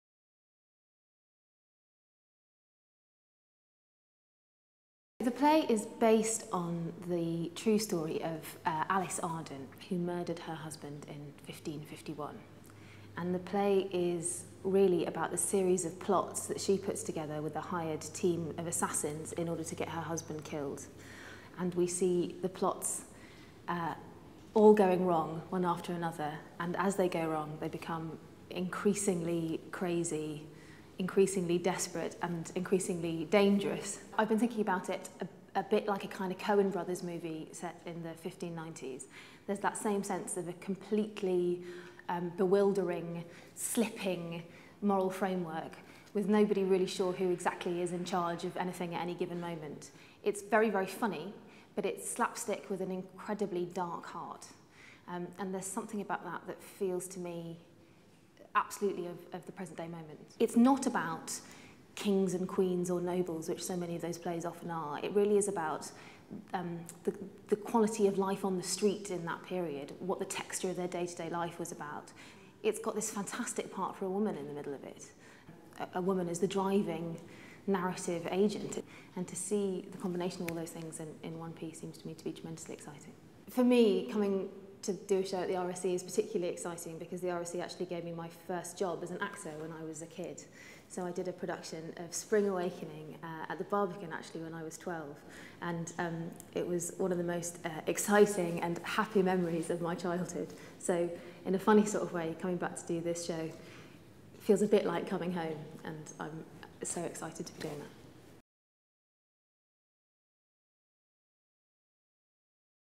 Interview with Polly Findlay | Summer 14 | Royal Shakespeare Company
Polly Findlay describes Arden of Faversham, and her early introduction to working at the RSC.